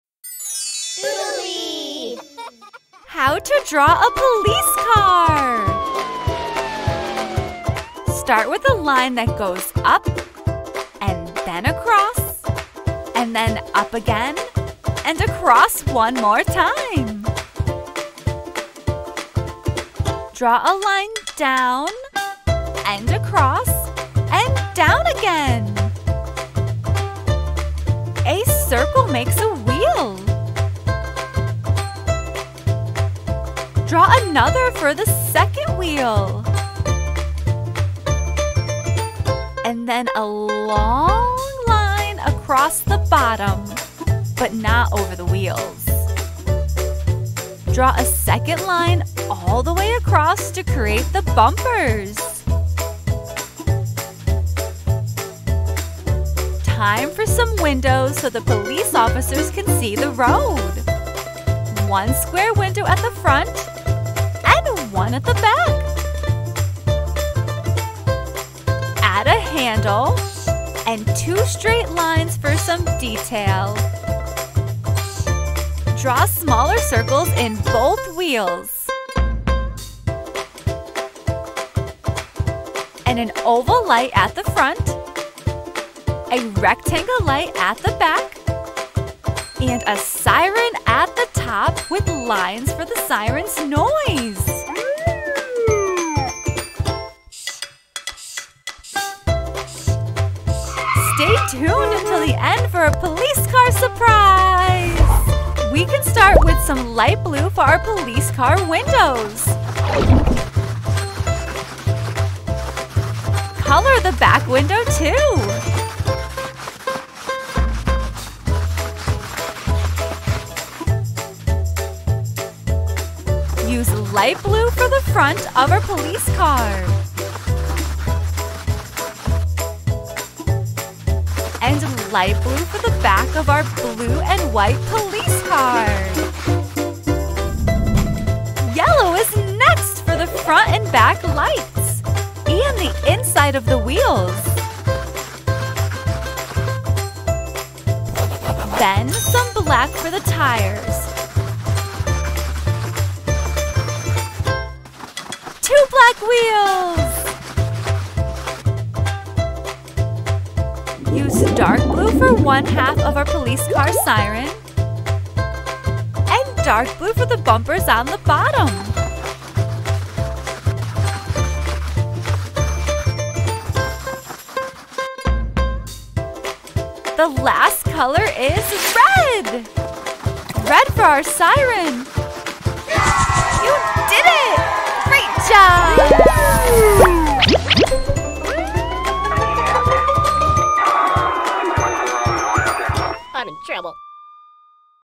How to Draw a Police Car – Fun & Easy Step by Step Tutorial | With Coloring | For Kids & Beginners
Learn how to draw a police car with our fun and easy step-by-step guide.

You’ll need:
- Permanent black marker (“Sharpie”) for drawing the police car
- Colored pencils in light blue, yellow, black, blue, and red for coloring the police car

This video is perfect for kids (and beginners) who want to learn how to draw, color, and create simple works of art! The lessons are easy to follow and fun for children of all ages.

This fun and educational video is great for toddlers, too! The fun animations, cute sounds, and cheerful music will keep them engaged, while they learn about colors and art.